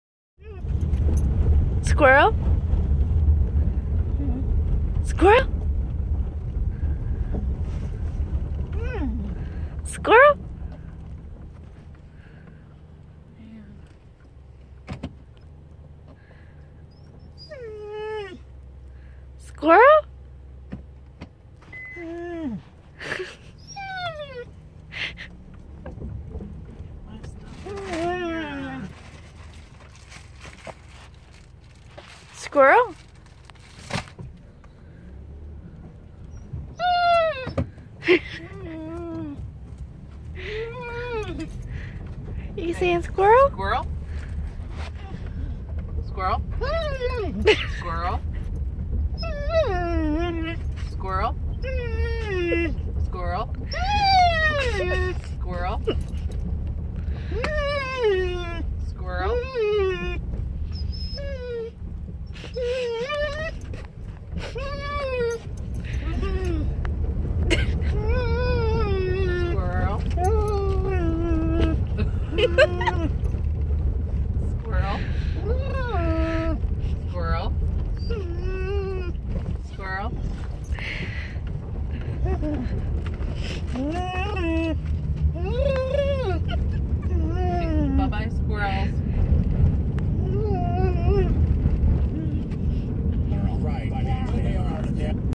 Dog saying squirrel
My dog Maggie loves squirrels and she makes this cute noise when she sees them